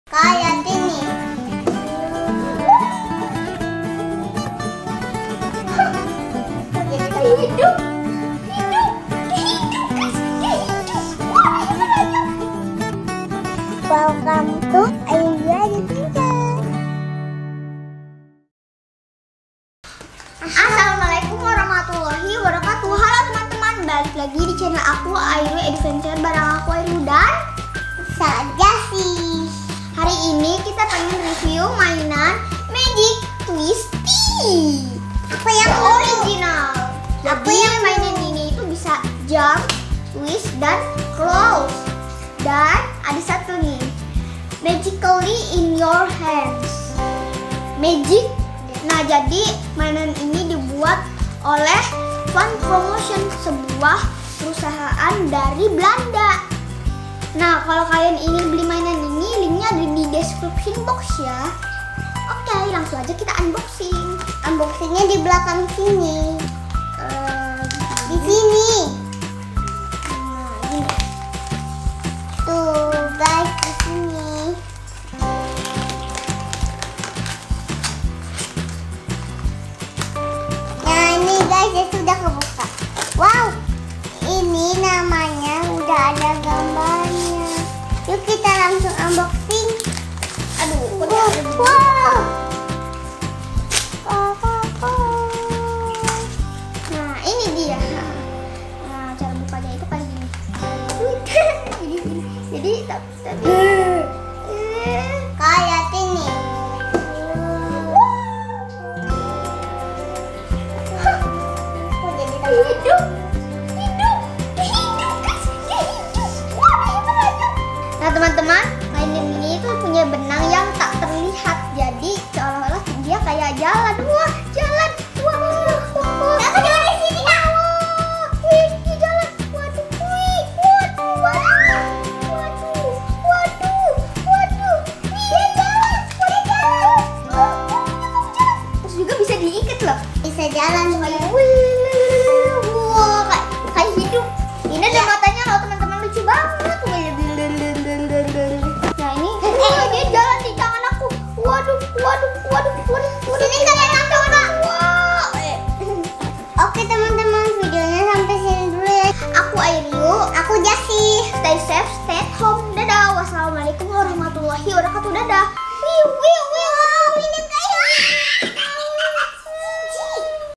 Airriu Beli Mainan Magic Twisty
kali ini airriu mau review mainan Magic Twisty dari Fun Promotion, seseru apa mainan ini?? tonton videonya yaa..